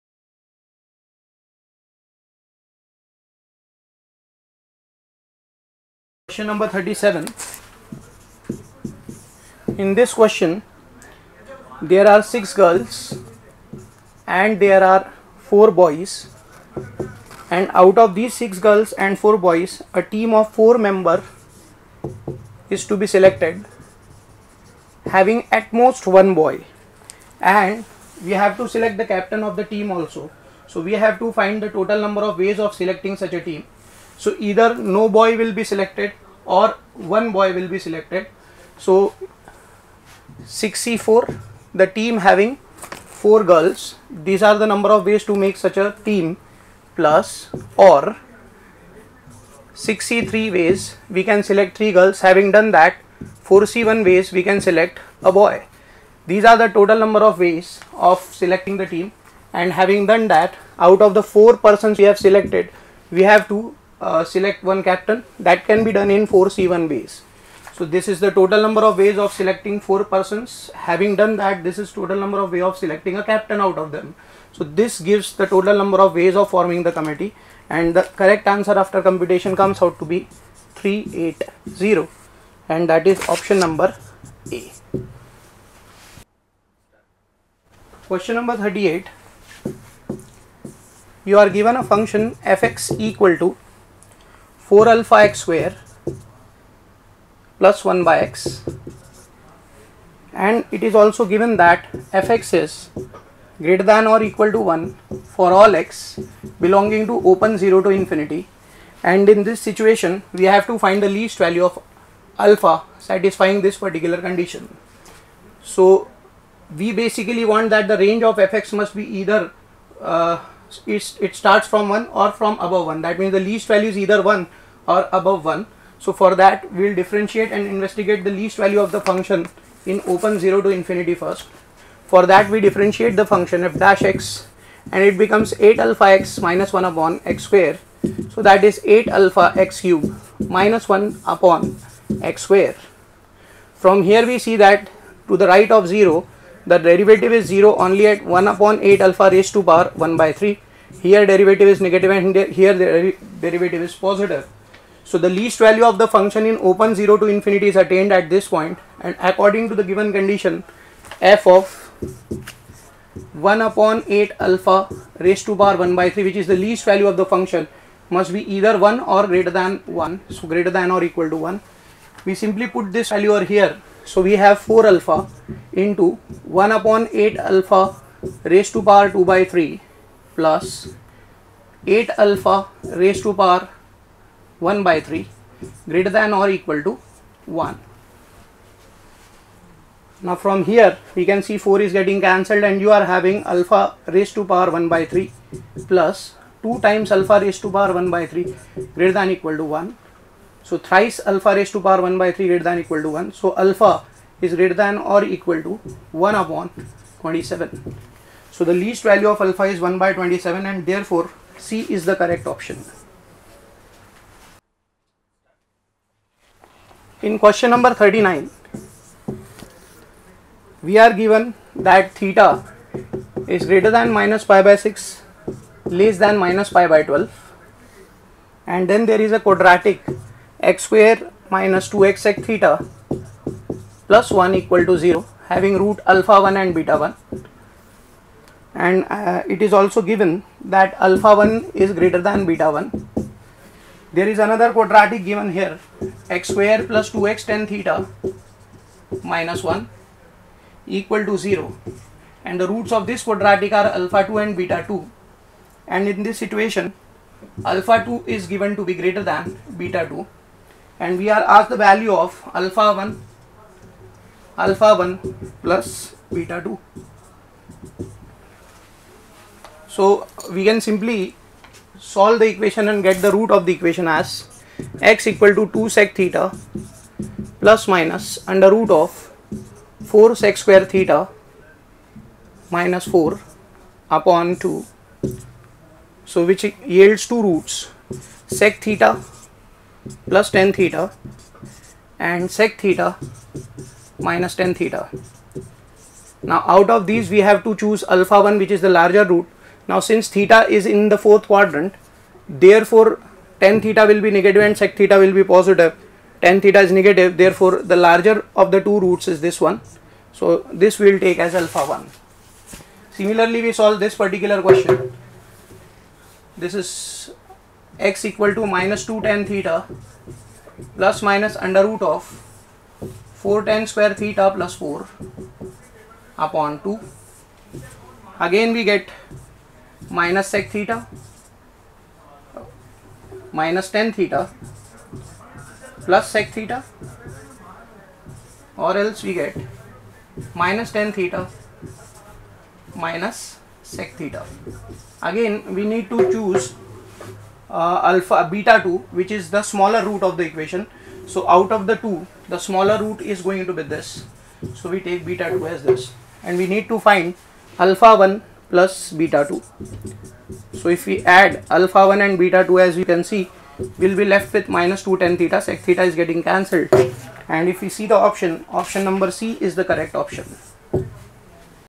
JEE Advanced 2016 Maths Solution Q. 37, 38, 39 (Paper-1) Code-9
Find IIT-JEE Advanced 2016 Maths Video Solutions for Q.No. 37, 38, 39 (Paper-1) code-9 by ALLEN CAREER INSTITUTE.